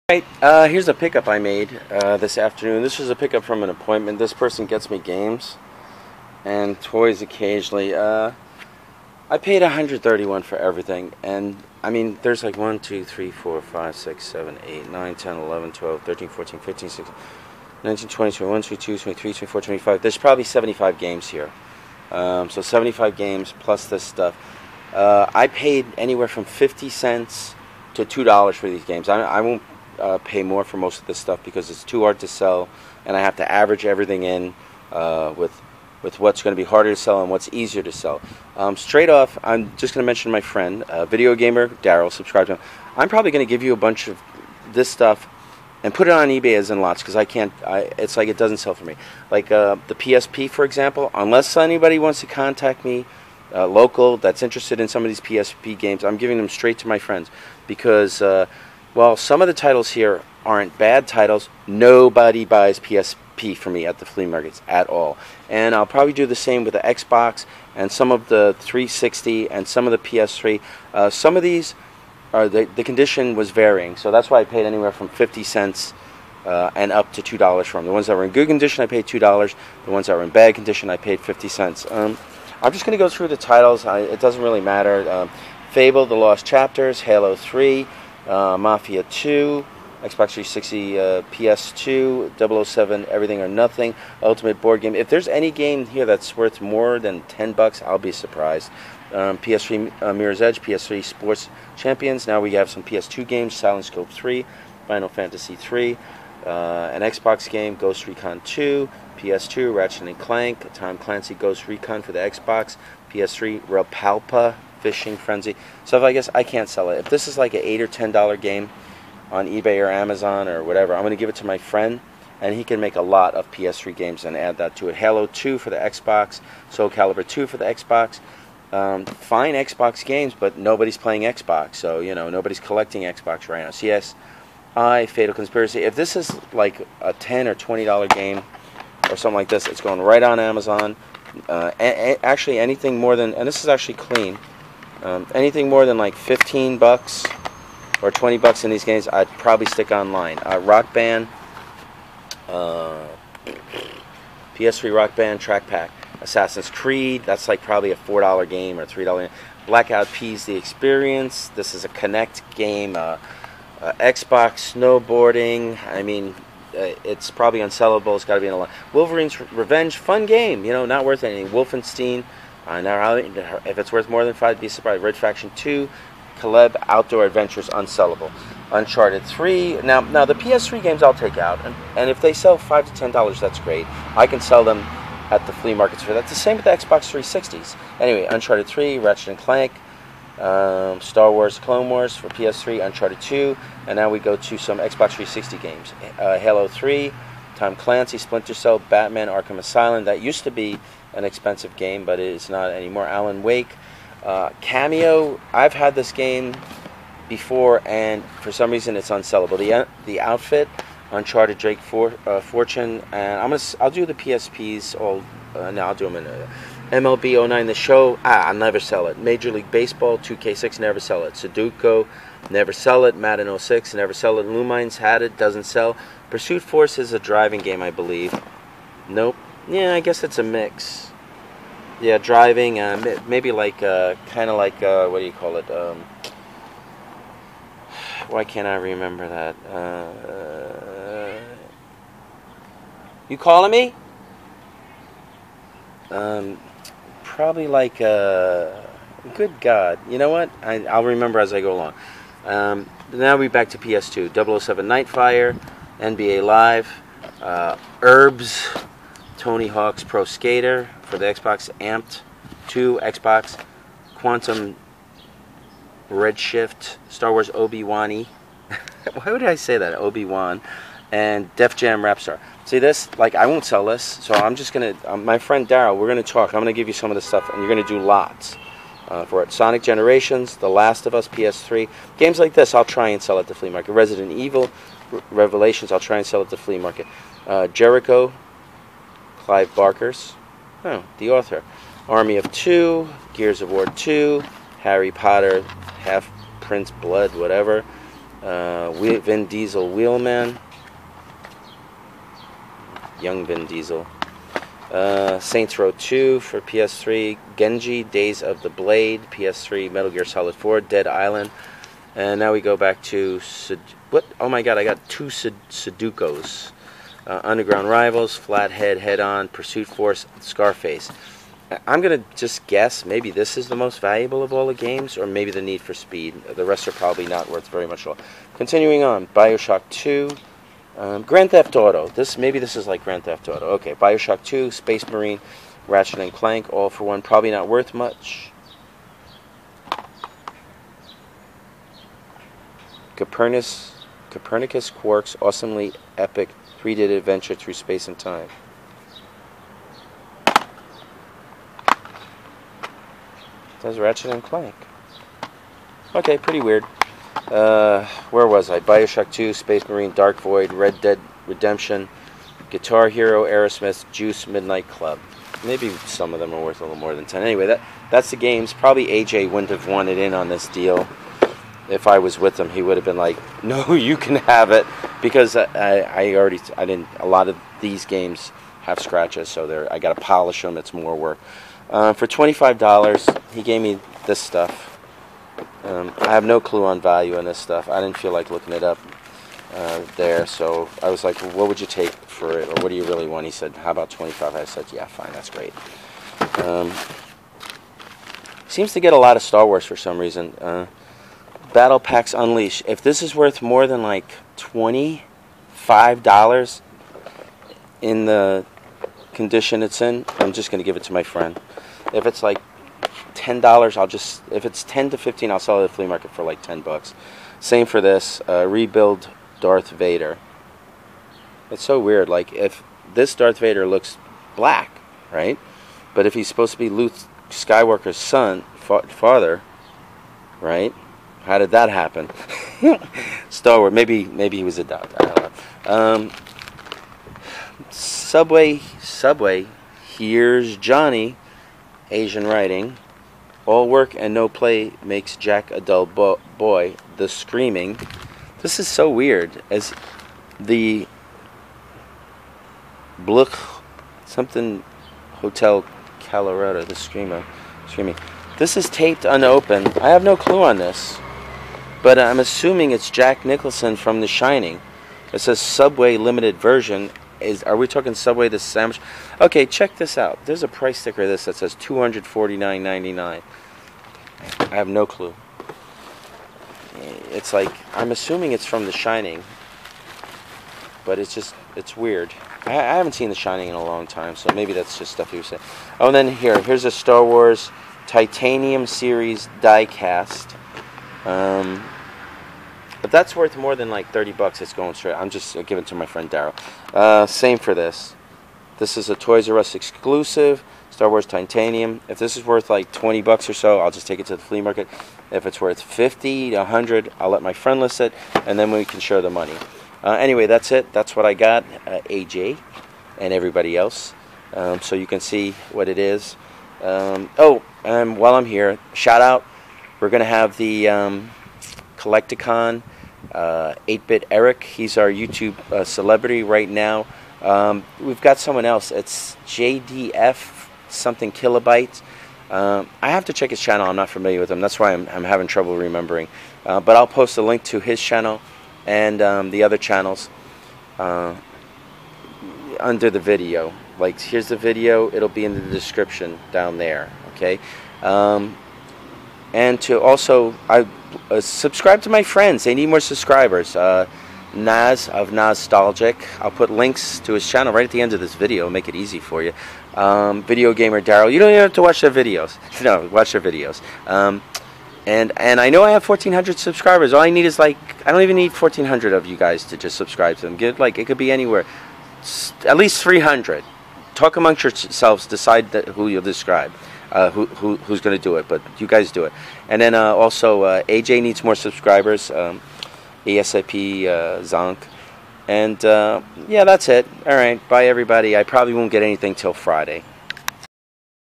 Video Games Star Wars Toys. Pick Ups from an Appointment - 5/1/17
Collect-A-Con Facebook

Here's some Games and Toys that I got from an appointment today. :-)

27-131